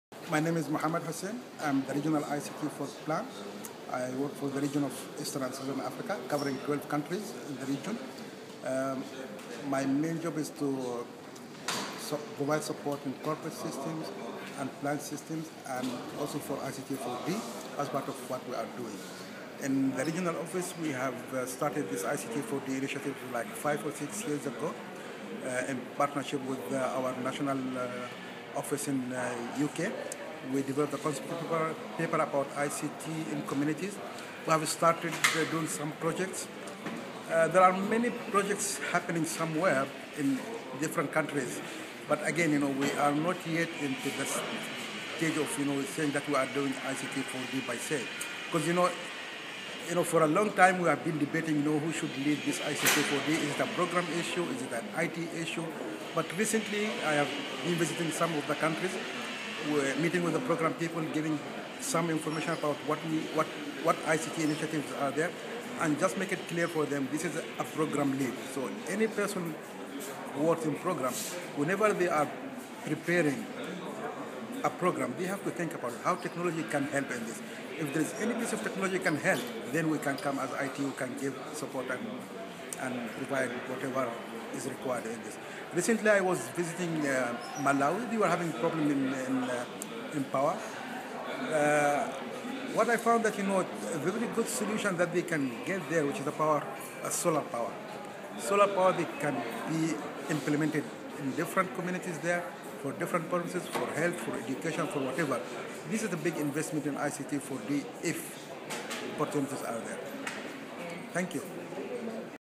ICT4E Realities - Mohamed Hussein Plan East and Southern Africa Region
Mohamed explains that program teams should lead the process and consider in each case if there is any technology that can support program goals. ICT staff should be there to support .